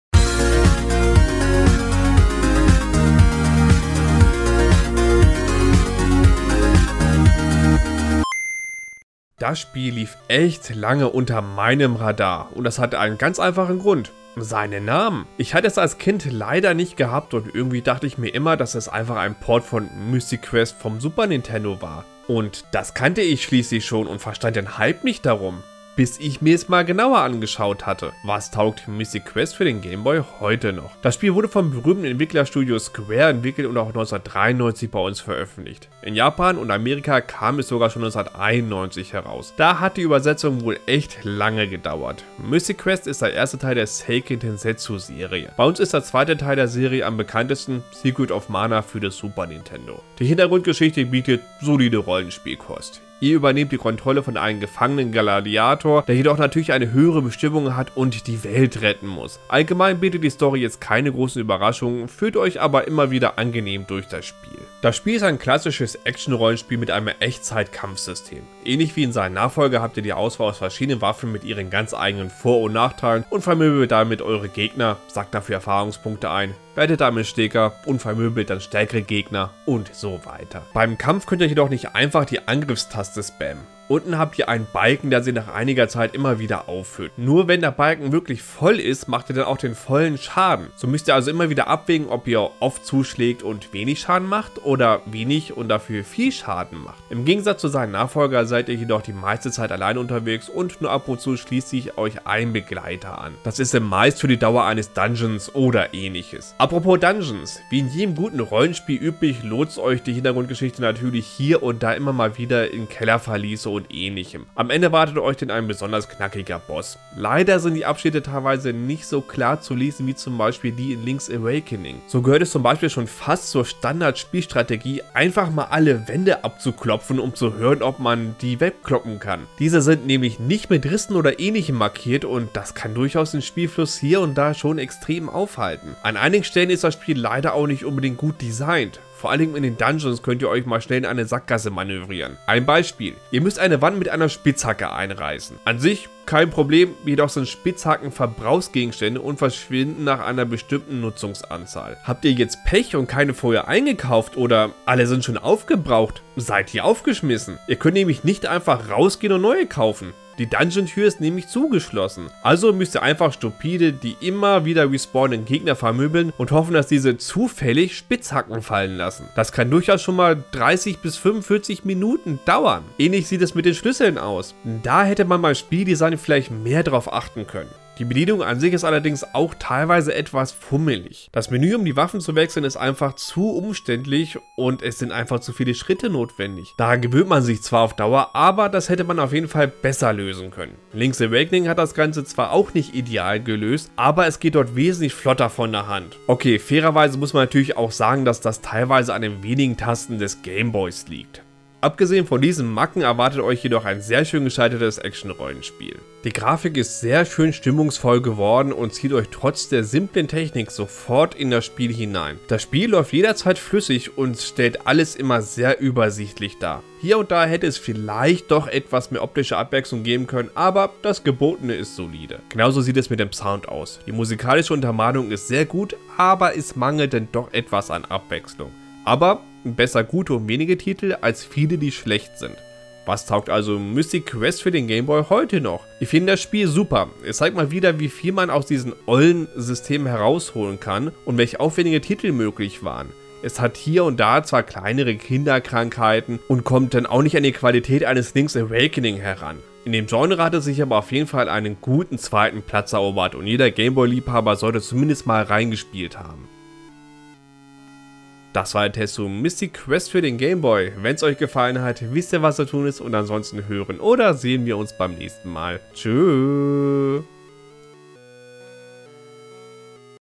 Was taugt Mystic Quest (Game Boy) heute noch? (Review/Test)
Damals ist der Vorgänger von Secret of Mana komplett an mir vorbei gegangen! Ich dachte immer es wäre einfach ein Port vom SNES Mystic Quest! In meinem LP wurde ich eines besseren belehrt. Wie gut ist es?